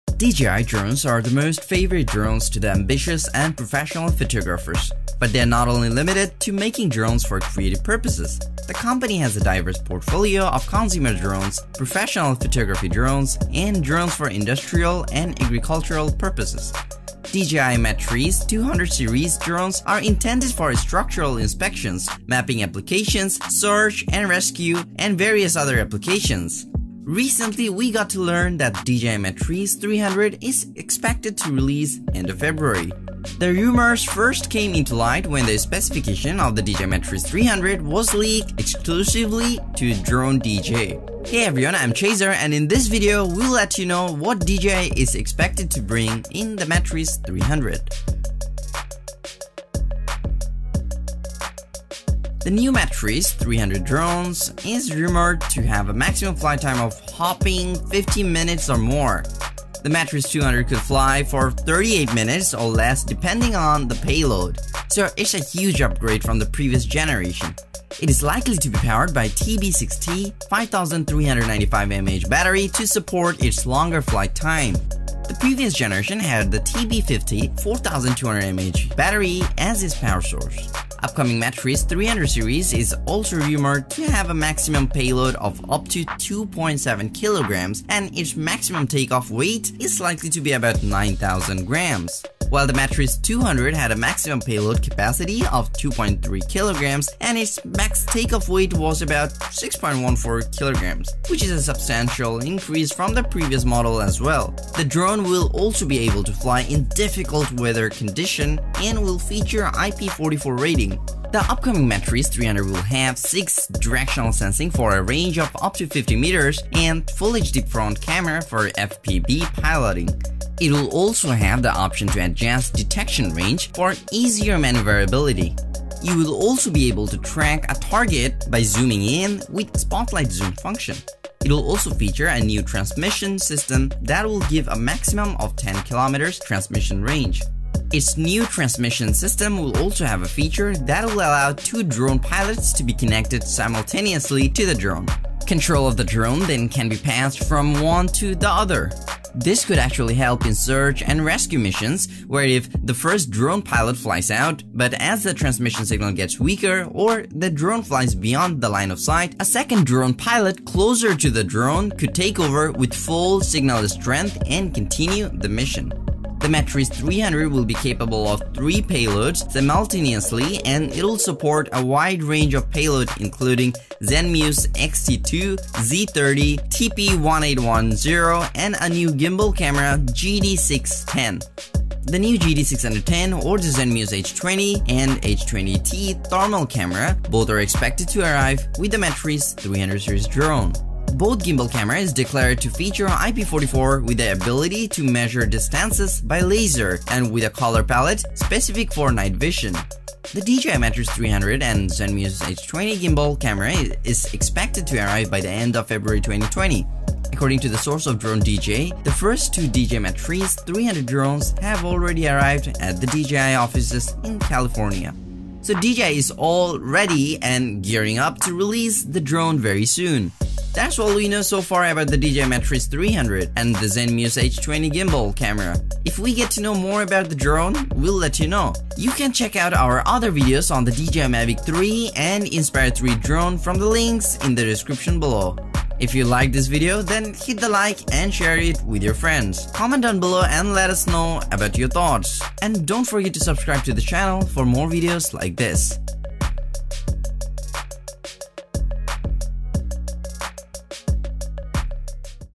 DJI MATRICE 300 Drone Final Leaks & Release Date
DJI drones are the most favorite drones to the ambitious and professional photographers. But they’re not only limited to making drones for creative purposes. The company has a diverse portfolio of consumer drones, professional photography drone and drones for industrial and agricultural purposes. DJI Matrice 200 series drones are intended for structural inspections, mapping applications, search and rescue, and various other applications. Recently we got to learn that DJI Matrice 300 is expected to release end of February.

***Best DJI Drone Deals on Amazon